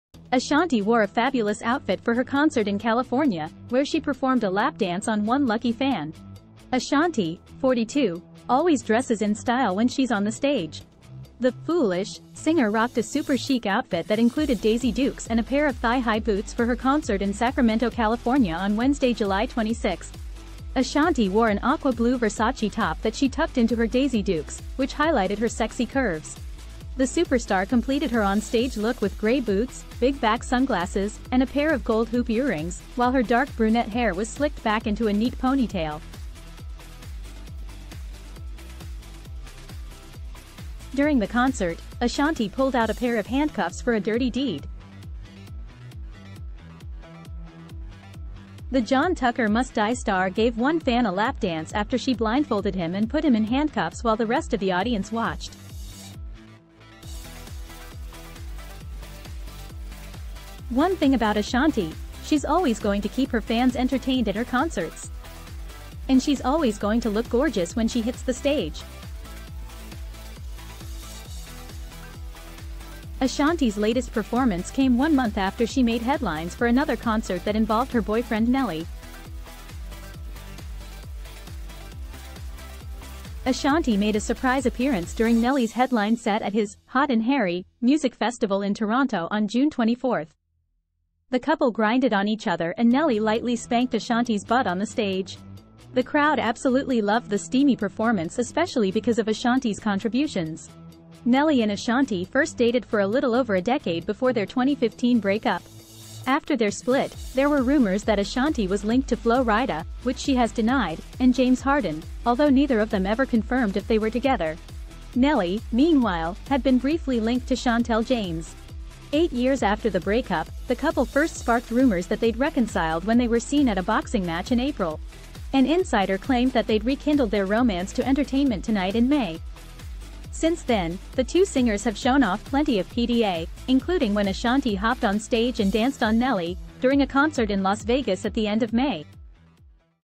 Ashanti Performs In Daisy Dukes & Thigh High Boots At Sacramento Concert
In a scintillating and unforgettable video, music enthusiasts are treated to a spectacular performance by the multi-talented Ashanti, who graces the stage in a jaw-dropping ensemble of Daisy Dukes and thigh-high boots during a thrilling concert in Sacramento. Join us in this captivating and high-energy video as we witness Ashanti's sensational artistry, dynamic stage presence, and daring fashion choices that set the crowd ablaze.
From the opening frames, the video immerses viewers in the electric atmosphere of the concert. Ashanti's sultry silhouette makes a bold entrance, adorned in a breathtaking combination of Daisy Dukes that flaunt her toned legs and thigh-high boots that exude confidence and style. The stage lights up with a riot of colors, and the audience erupts in cheers, eagerly anticipating the show-stopping performance that is about to unfold.
In this video, music critics and fans alike delve into Ashanti's remarkable talent and undeniable star power. They analyze her vocal prowess, her ability to command the stage, and the captivating aura she brings to each performance, underscoring her status as a true music icon. Their insights add depth and context to the unfolding story, allowing viewers to appreciate the magnitude of Ashanti's artistic brilliance.
The video's cinematography captures the electrifying energy of the concert. Close-ups reveal Ashanti's magnetic presence as she effortlessly engages the audience, and the crowd responds with adoration and awe. The camera skillfully captures every dance move, every high note, and every mesmerizing moment, immersing viewers in the heart of the music magic.
The accompanying soundtrack enhances the enchanting experience, featuring Ashanti's mesmerizing vocals and the exhilarating beats of her chart-topping hits. The music builds up, perfectly complementing the show-stopping outfit and electrifying ambiance, evoking a sense of excitement and euphoria among both the concertgoers and viewers at home.
Beyond the captivating fashion and captivating performance, this video celebrates the essence of artistic expression and the transformative power of music on stage. It emphasizes the importance of pushing creative boundaries and embracing fashion as a powerful form of self-expression. It inspires viewers to embrace their individuality and to bask in the magnetic allure of Ashanti's artistry.
While celebrating Ashanti's daring fashion choices and electrifying performance, the video maintains a respectful and celebratory approach, aiming to provide an informative and inspiring account of this unforgettable concert moment. It invites viewers to share in the excitement of witnessing a true music icon in her element, while still honoring the privacy and professionalism of the artist.